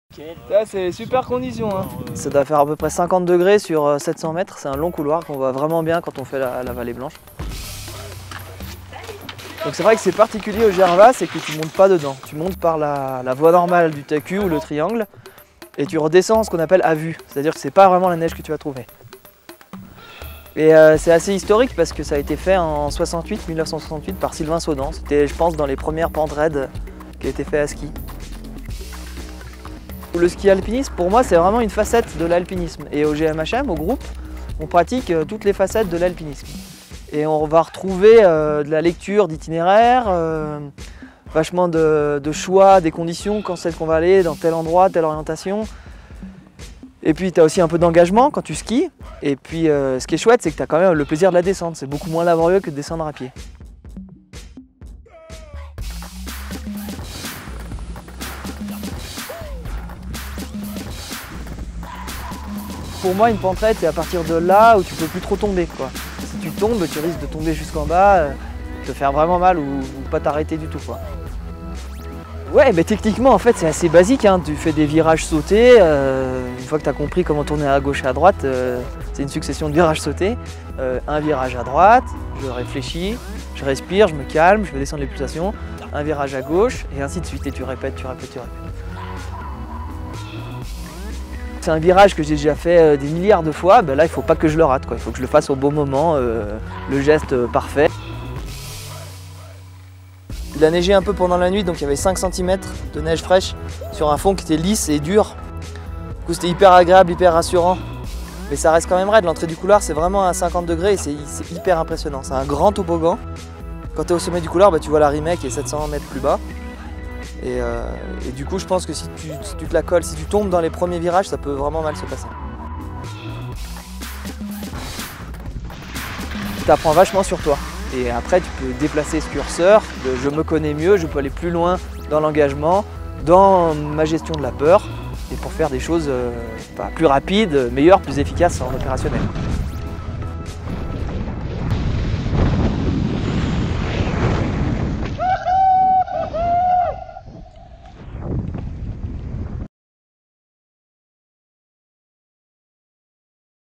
2018TGMHM05Gervasutti
Le GMHM profite des bonnes conditions en ski de pente raide. Pour accéder au couloir Gervasutti en face Est du Mont-Blanc du tacul, il nous a fallu monter par Helbronner puis traverser jusqu'au refuge des cosmiques. Quel plaisir simple d'être en montagne, faire quelques virages et ramener de belles images.